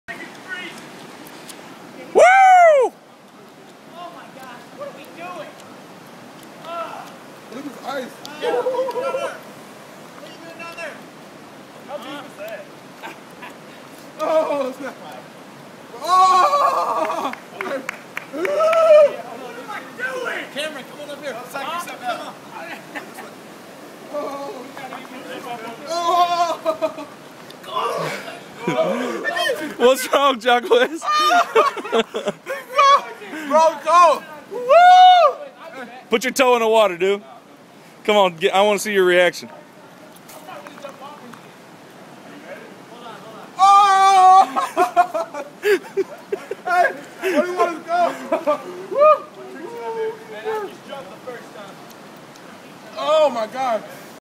Polar Bear Swim 2011 VLOG #3
It is cold. It took everyone a little bit to talk themselves into the stupidity of it all. Haha. Jaquise was not ready for how cold the water was. Ha.